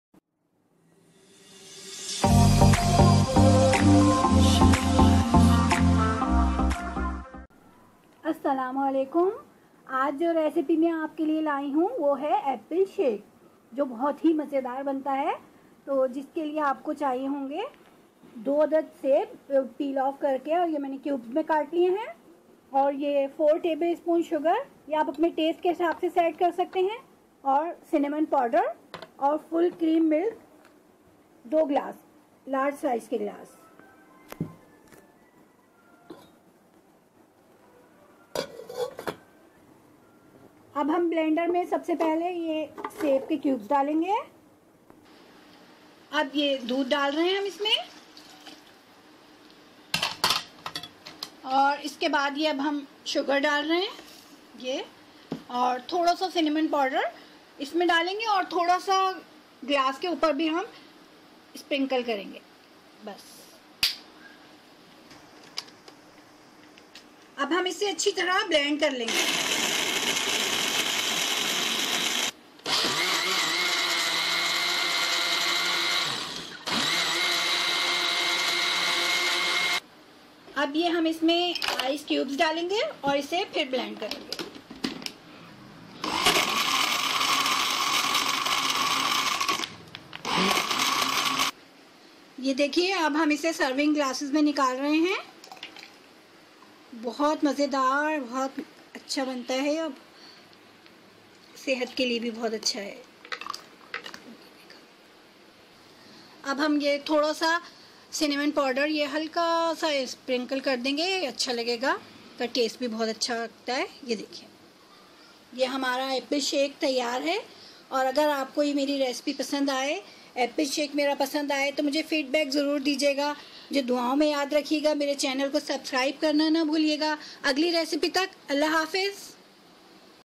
Apple milk shake
Ingredients:

Apple:(2)

Milk:(2) glass

Cinimon:1/4 tea spoon